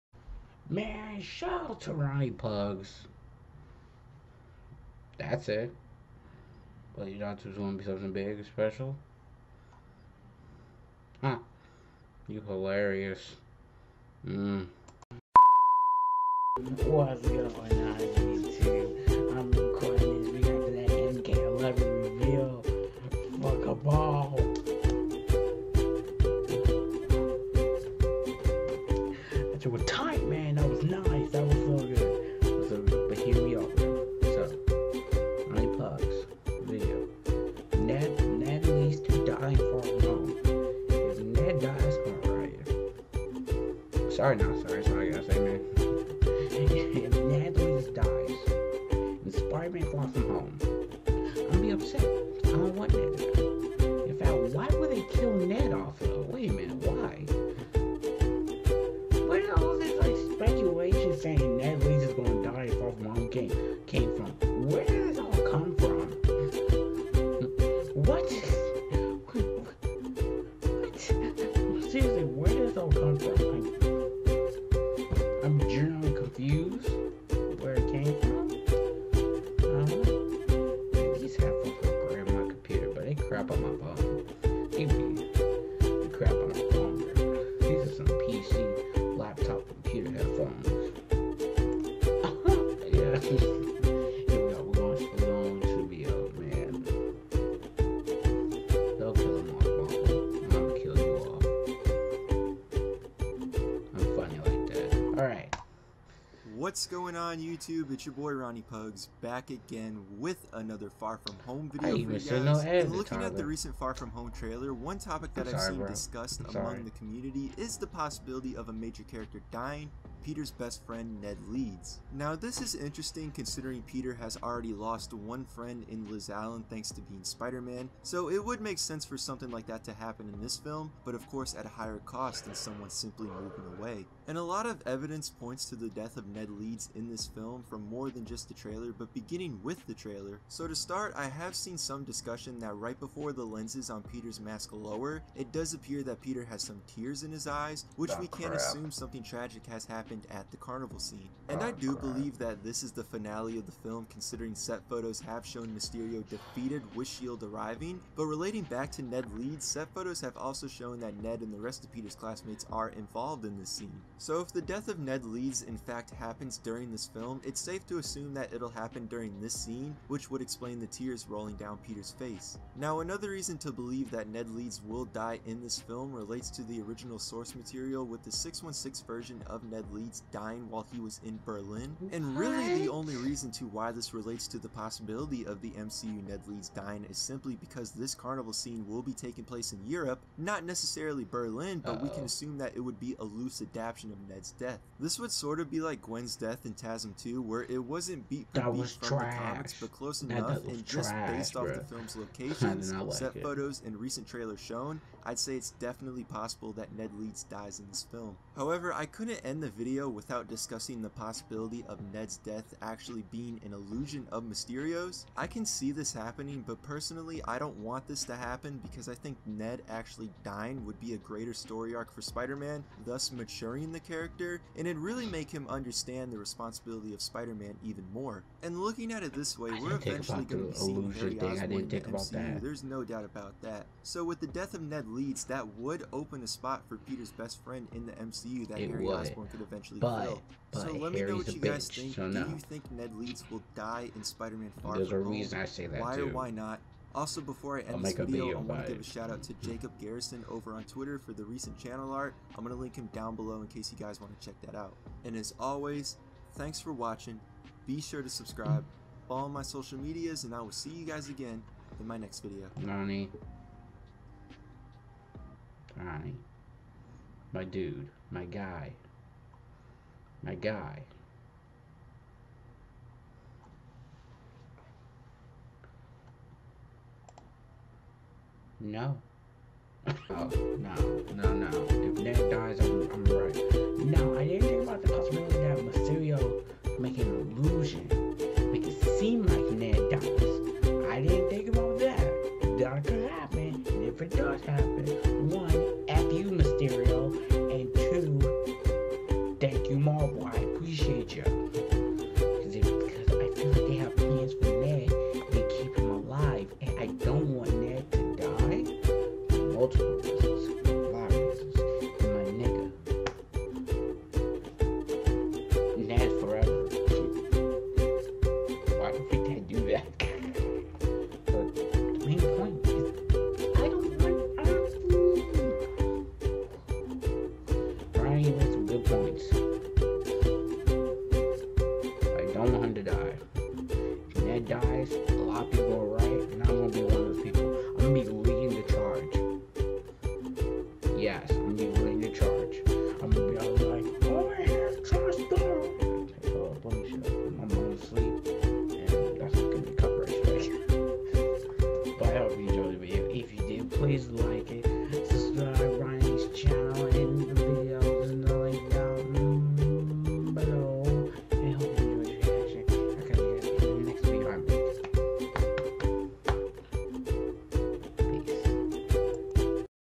'Ned Leeds to DIE in Spider-Man: Far From Home?' REACTION!!! (i hope not...)
S/O to Ronny Pugs!!! He's a good guy so check out his video and channel too.

🔔CLICK that bell for notifications as well!